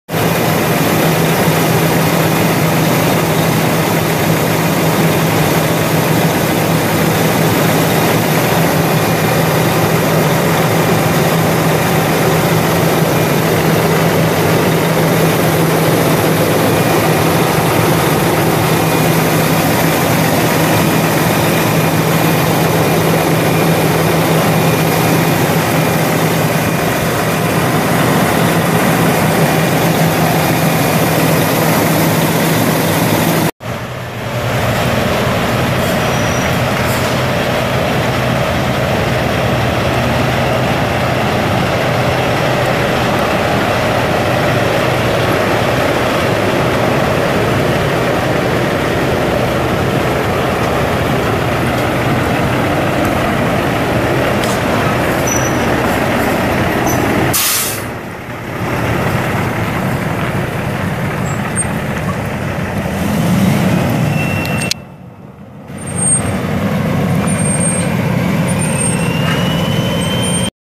2005 International 9400i semi truck for sale at auction | bidding closes February 4, 2021
2005 International 9400i  semi truck
1,159,709 miles on odometer
Engine
 Cummins  ISX
 Displacement: 14.9L
 Cylinders: 6
 Fuel type: Diesel
Transmission
 Eaton Fuller
 Manual
 Speed: 10
Chassis
 Axles: Tandem
 Differential lock: Inter-axle
 Suspension: Air
 Brakes: Air
 GVWR: 52,000 lbs
Interior
 AC, Heat
 Heated mirrors
 Power windows, Power locks
 Cruise control
 Air ride cab
 Beds: Double bunk
 Rear climate control
Operational
 Deck plate: Aluminum
 Fifth wheel type: Sliding
 Fuel tank: Dual
Tires
 Front: 11R22.5
 Rear: 285/75R24.5
Missouri title
2HSCNAPR55C146121
It's located in Princeton, MO, and we're selling it to the highest bidder regardless of price.